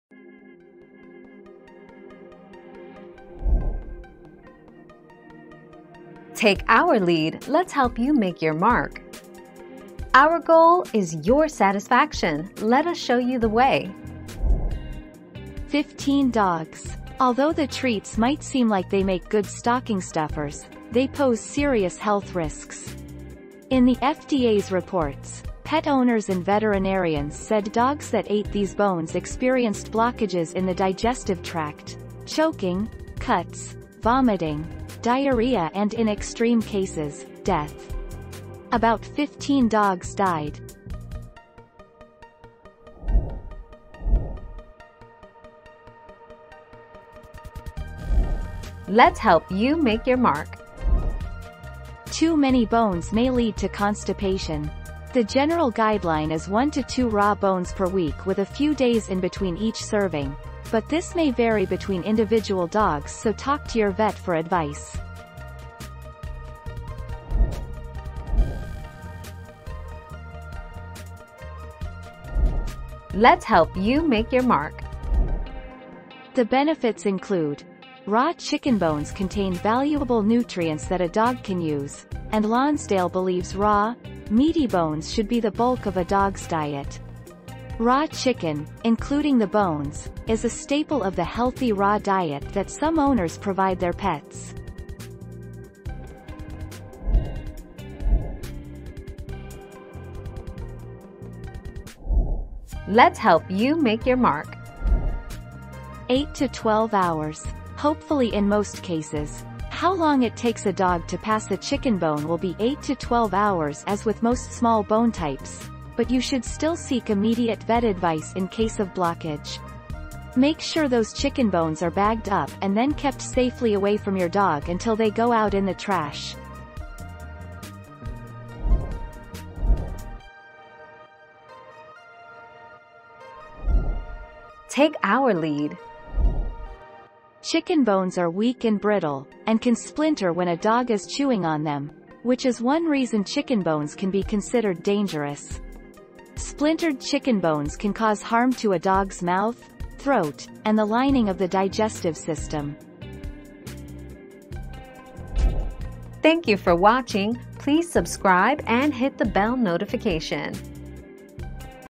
How many dogs have died from eating chicken bones?
00:00 - How many dogs have died from eating chicken bones?
00:45 - How many bones can a dog eat a day?
01:09 - Why do dogs love chicken bones?
01:38 - How long does it take for a dog to pass a chicken bone?
02:10 - Why chicken bones are bad for dogs?

Laura S. Harris (2021, May 15.) How many dogs have died from eating chicken bones?
    AskAbout.video/articles/How-many-dogs-have-died-from-eating-chicken-bones-245081